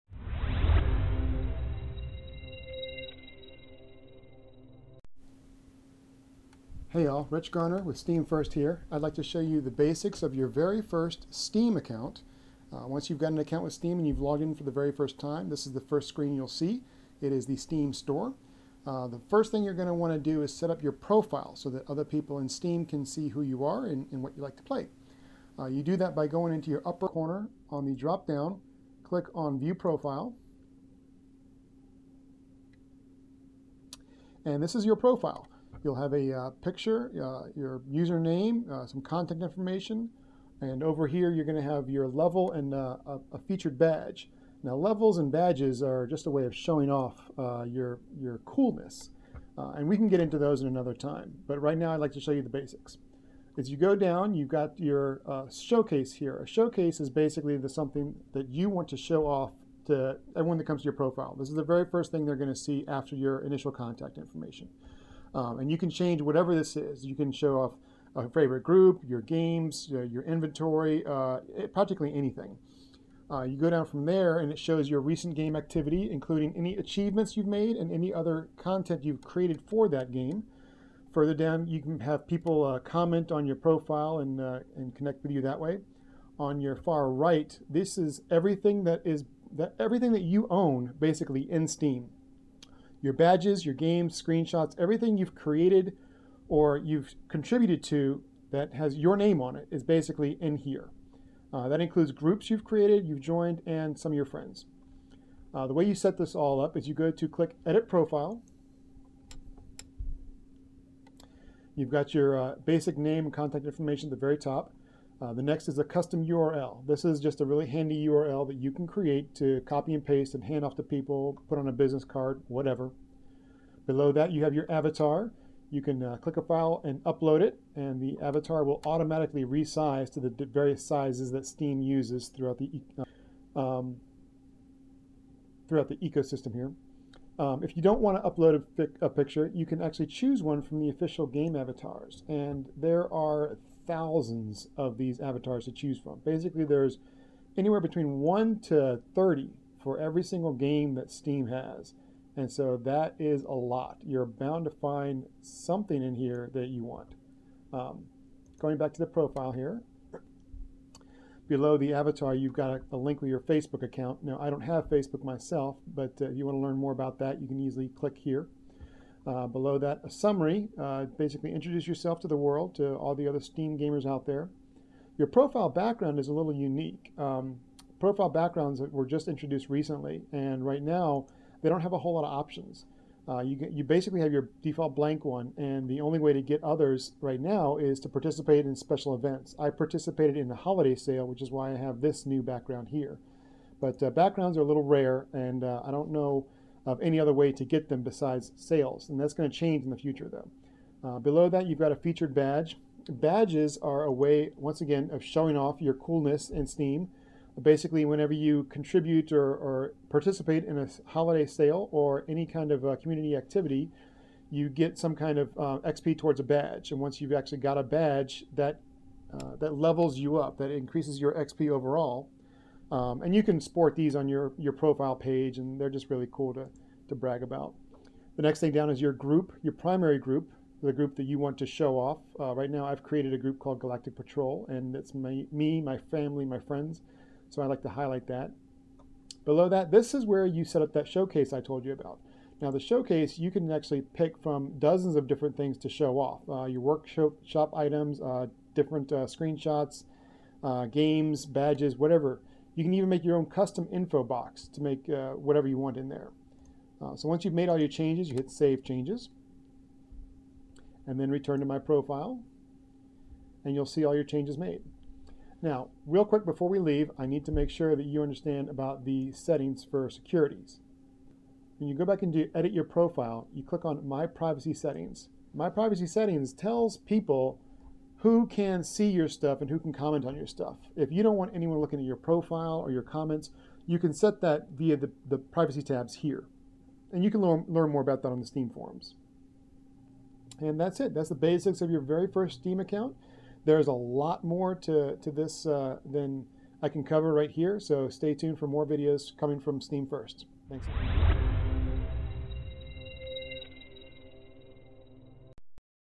How To Set Up Your Steam Profile - SteamFirst
All this press about SteamOS and Steam Machines is generating a lot of interest for our favorite gaming service. Seems I'm finding friends join that I never would have labeled as gamers.

We here at SteamFirst are thrilled to see new Steamers and we want to help you get the most of your Steam experience. So we are preparing a series of short videos that cover every aspect of Steam (besides the actual games).

First up: A quick intro to setting up your Steam profile.